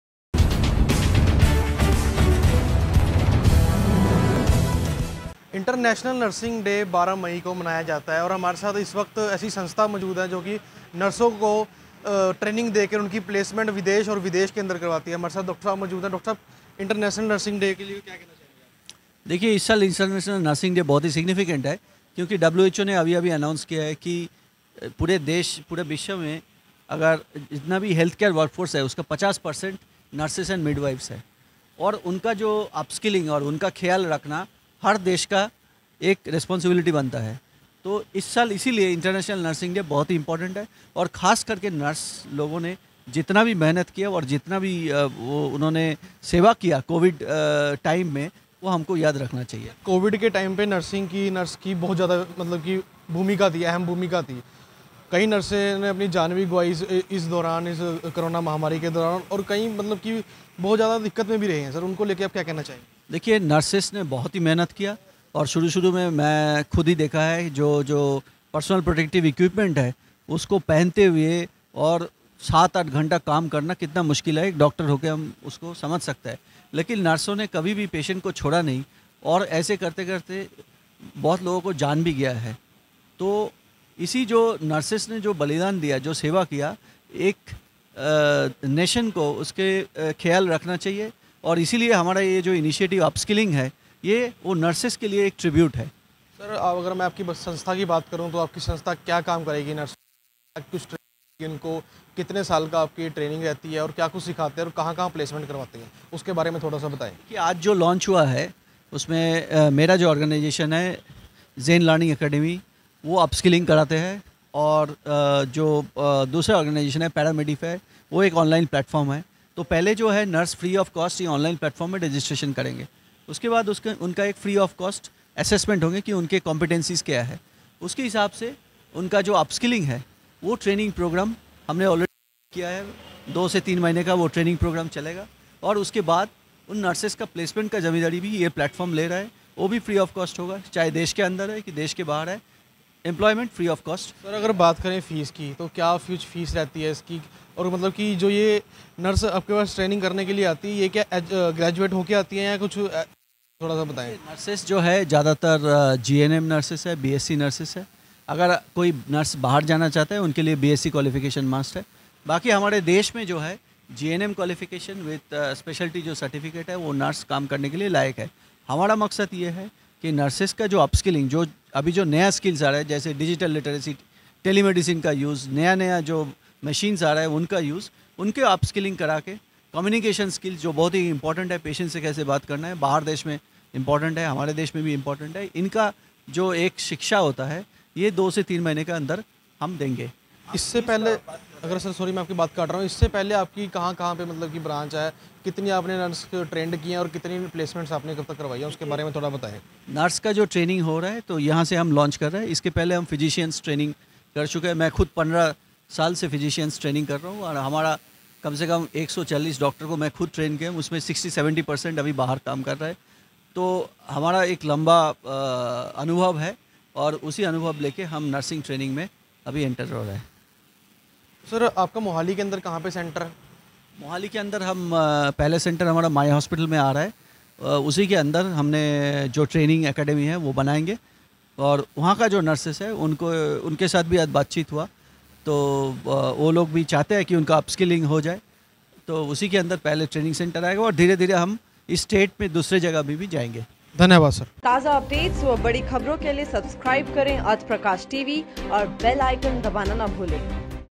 International Nursing Day पर डॉक्टर्स ने अर्थ प्रकाश से की खास बातचीत
International Nursing Day पर डॉक्टर्स ने अर्थ प्रकाश से की खास बात चित